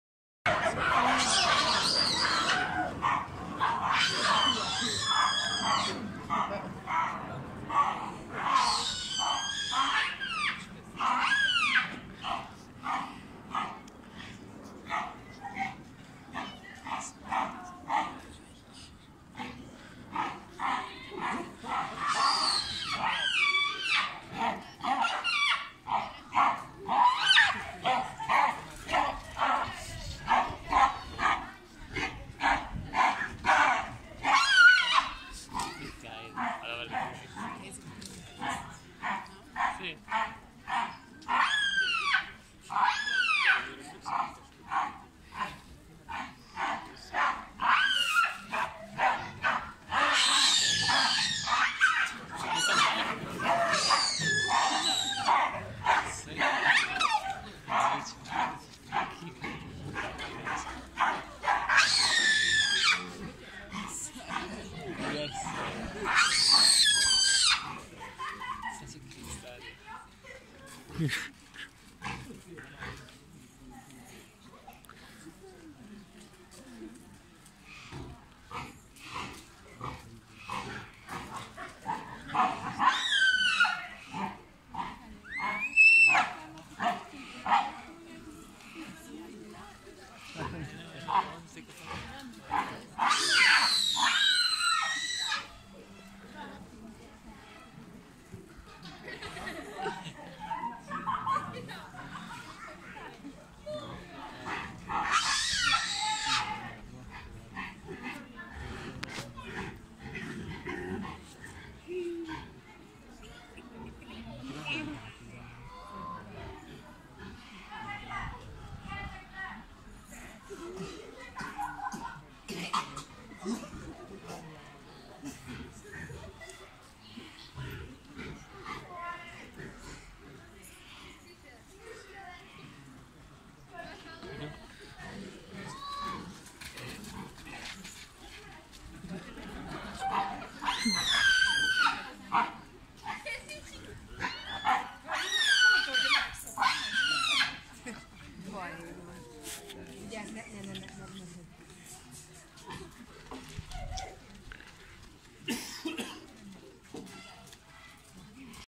Barbary sheep🐐vs Baboons 🙉
The baboons didn't like that the sheeps also want come down.